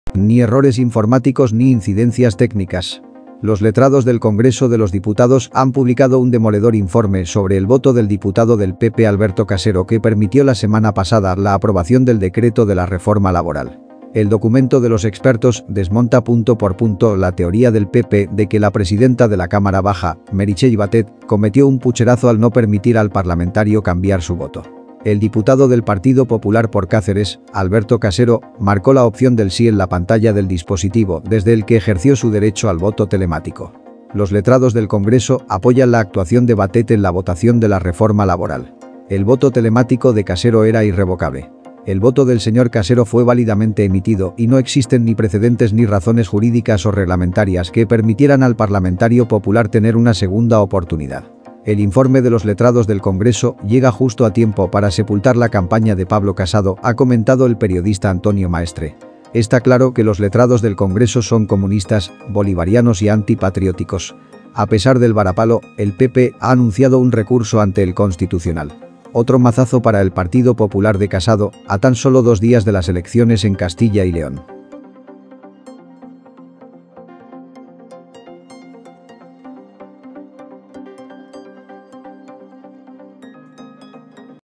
MAZAZO para PP: los letrados del Congreso DESMONTAN el 'PUCHERAZO' de Casado
¡Un saludo!